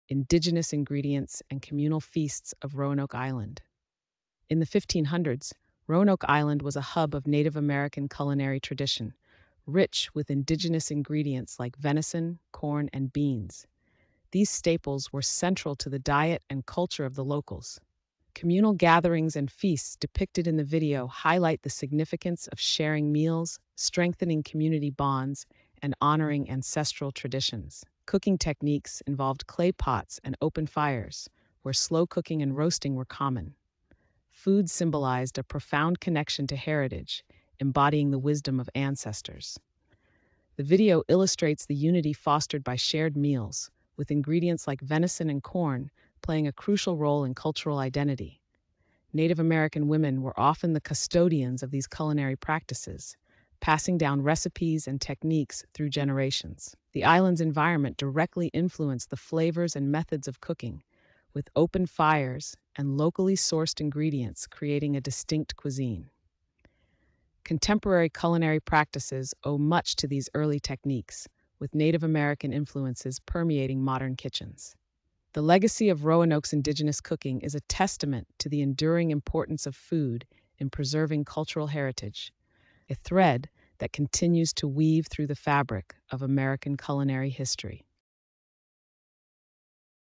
Can I Survive Cooking on Renoke Island?
Join us on a culinary adventure as we explore the traditional cooking methods of Renoke Island. Immerse yourself in the rich flavors and cultural heritage of this remote island as we witness locals preparing their traditional dishes using age-old techniques passed down through generations. From fresh seafood to exotic spices, experience the incredible diversity of ingredients that make Renoke Island cuisine truly unique. Don't miss out on this exclusive glimpse into the heart of traditional cooking on Renoke Island!